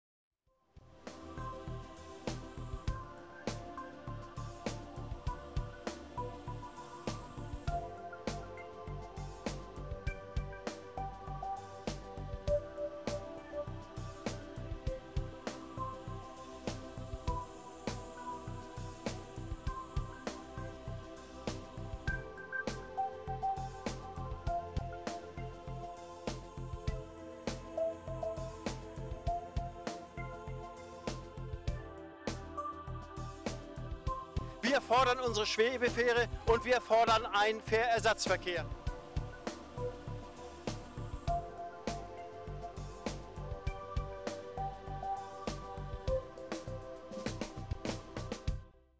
Erinnerungen an die Schwebefähre
..am 8.1.2016 lag tiefe Trauer über Rendsburg, dem Nord-Ostsee-Kanal und der ganzen Region, eine Havarie morgens zwischen einem Frachter und der Schwebefähre. Sie war nicht mehr zu erhalten und wurde demontiert. Ein Unglück, das sich hat niemand vorstellen können. In meinem neuen Film, der 33.10 Minuten lang ist, habe ich Bilder eingefangen von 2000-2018. Dieser neue Film wird, zum Filmfest, im Kino-Center Rendsburg am 4.3. gezeigt.